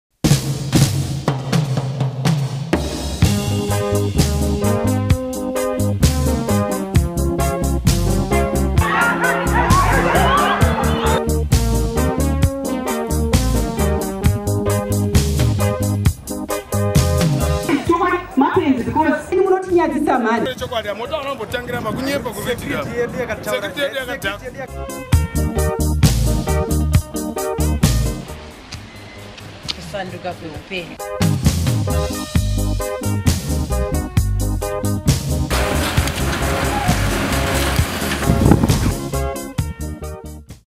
Zimbabwe's 2015 main events in 40 seconds
Mugabefalls, BringBackItaiDzamara and many more events of 2015 all presented in 40 seconds